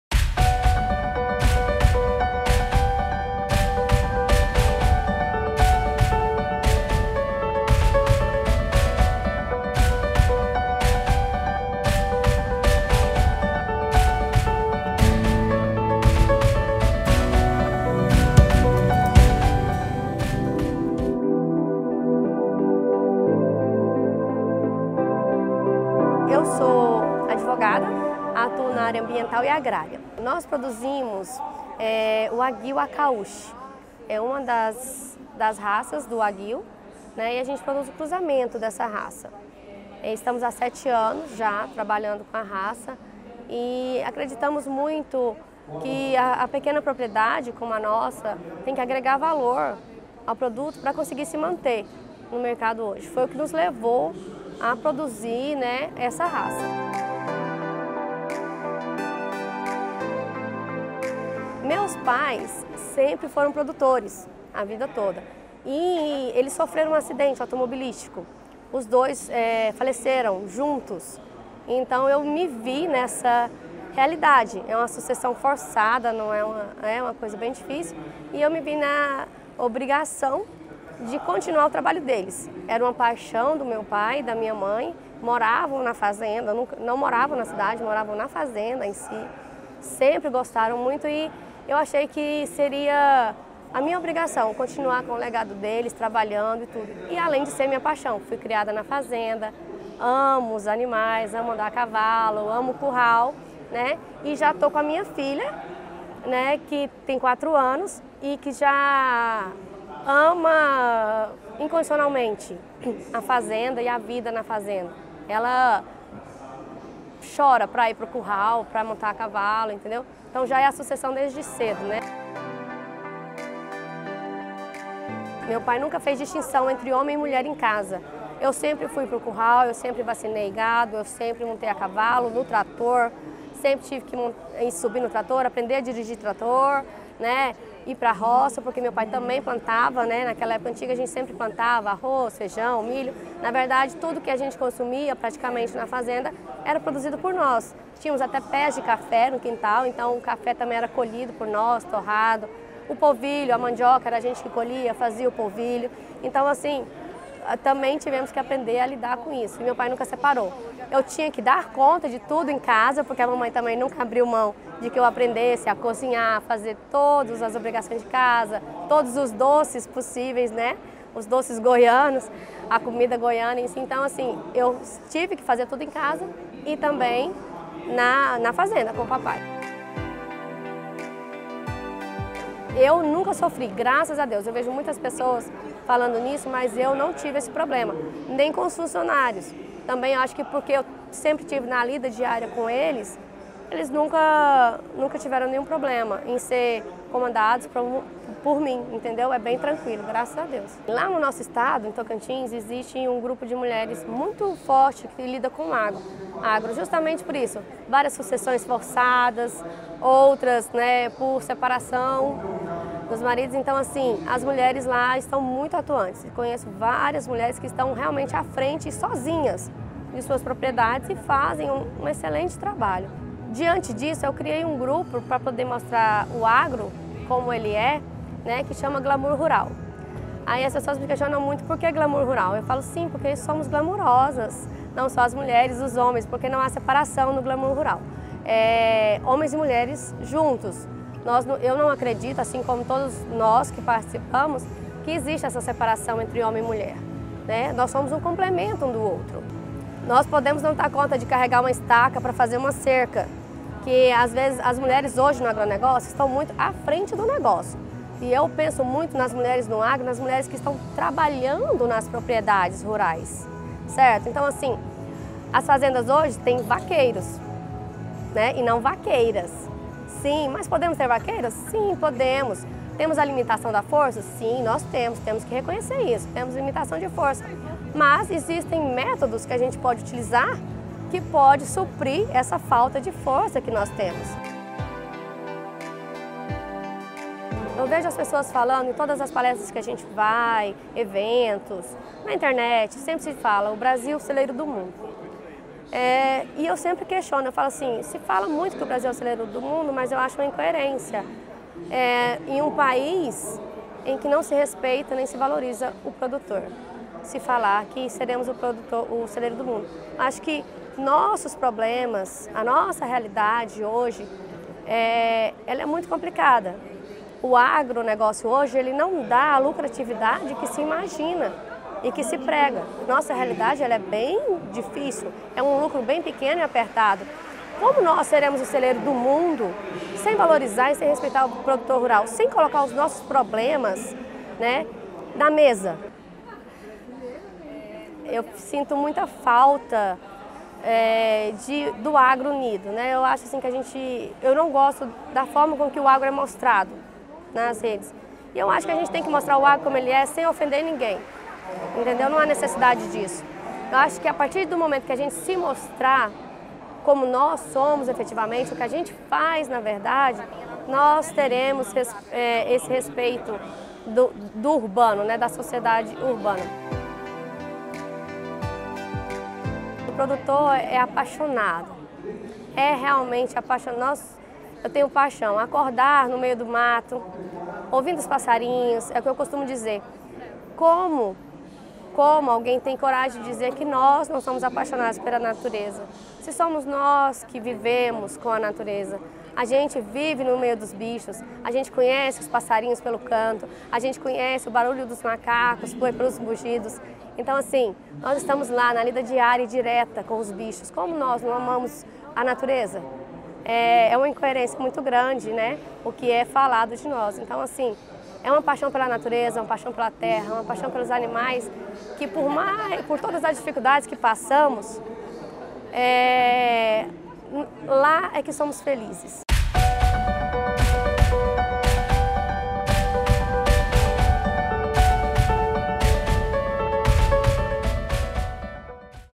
Mulheres: O Campo é delas!!! Ep 01 - Vivian Machado
Revista Rural no Congresso Nacional das Mulheres no Agronegócio Assista ao Programa Revista Rural, que traz tudo o que rola de mais importante no agronegócio: